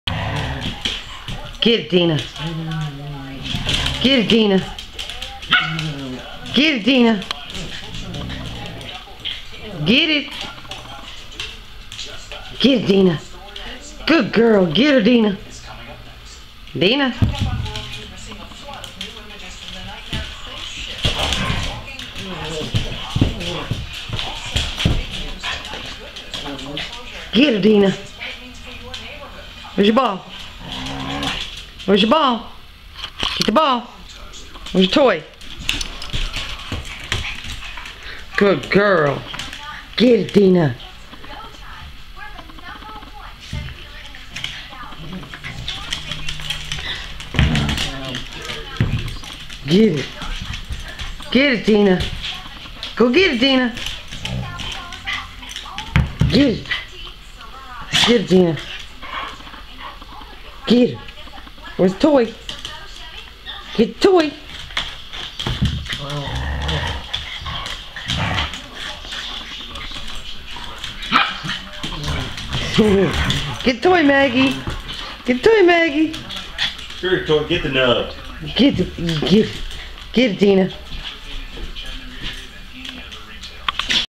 More Doggy Playtime
Maggie and Dina at it again!